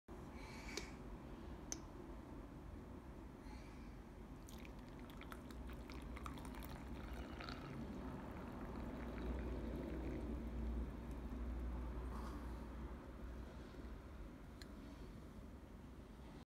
ハリオ 茶茶急須 丸 CHJMN-70T お茶を注ぐよ
ハリオ 茶茶急須 丸 CHJMN-70Tのレビューはこちら